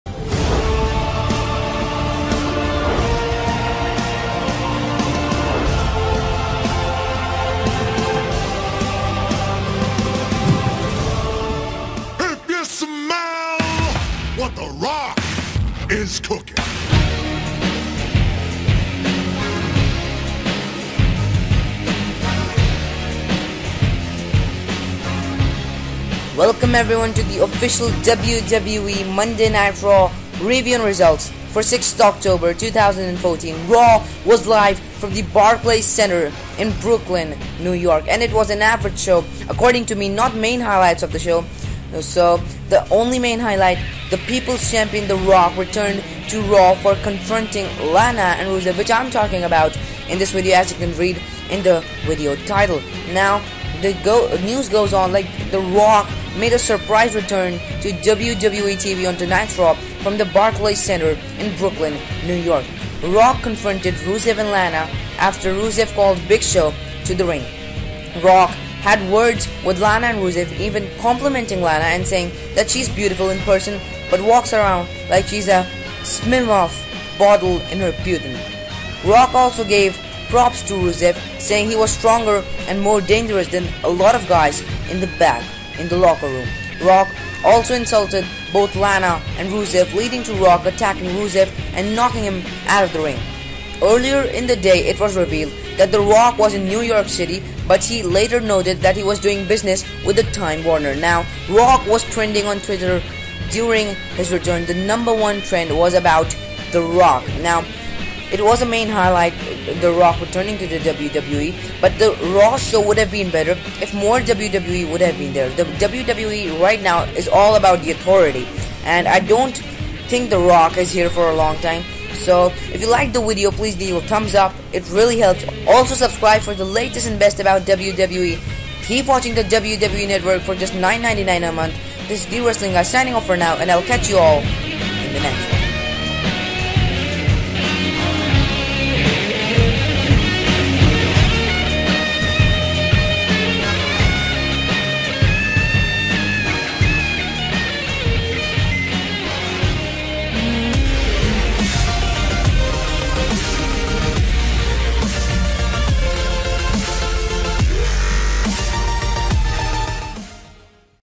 WWE MONDAY NIGHT RAW 6 OCTOBER 2014 (10/6/14) FULL SHOW REVIEW: THE ROCK RETURNS TO WWE!
WWE MONDAY NIGHT RAW 10TH OCTOBER 2014 (10/6/14) REVIEW: THE ROCK RETURNS!
➜SUB!
➜Follow on G+
➜The Wrestling Guy

➜Owner: TheWrestlingGuy
➜Nothing like downloading somebody else's video and uploading it has been done. All videos are Fresh and self-made

- THE WRESTLING GUY